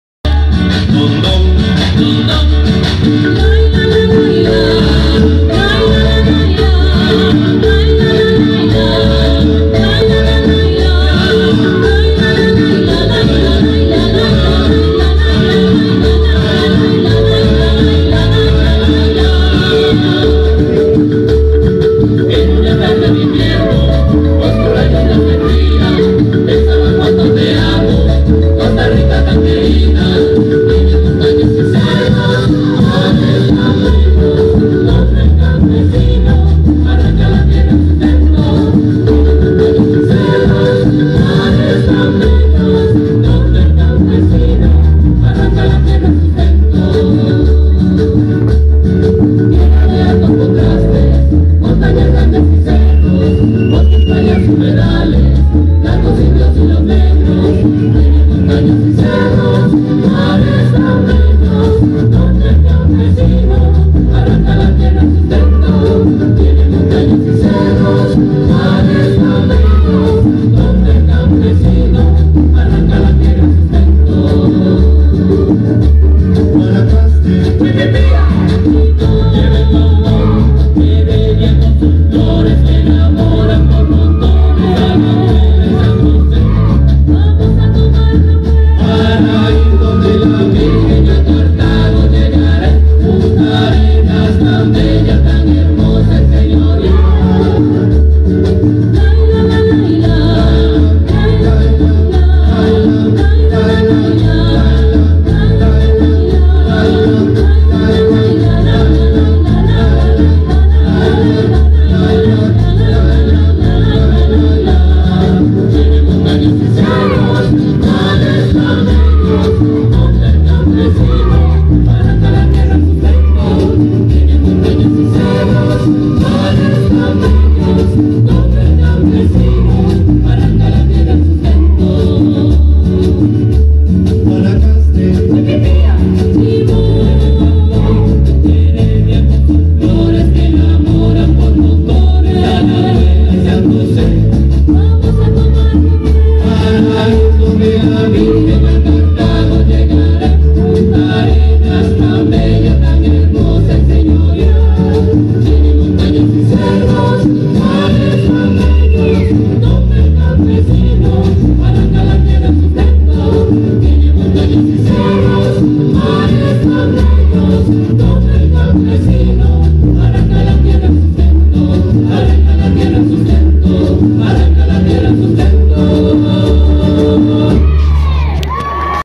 Folklore de Costa Rica
Interpretado en la Manzana del templo de la Ciudad de México. No cuento con los derechos de autor, para uso cultural sin fines de lucro.